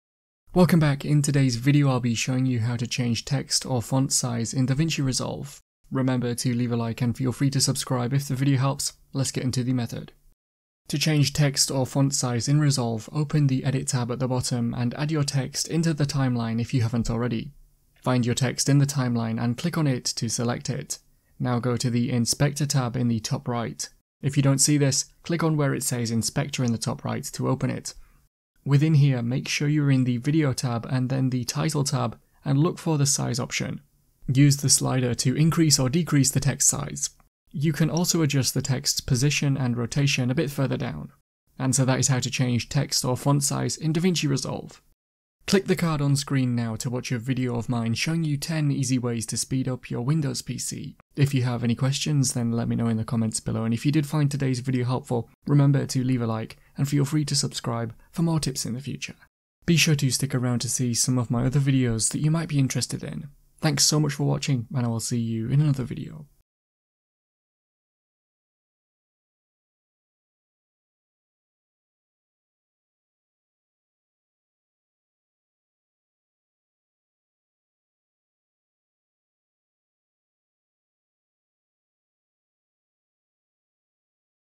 How to Change Font/Text Size in Davinci Resolve
Want to know how to change font size in Davinci Resolve? This video will show you how to change text size in Davinci Resolve. You might want to know how to adjust text size in Davinci Resolve to make text easier to see. Hopefully today's video helps show you how to increase text size or how to decrease text size in Davinci Resolve!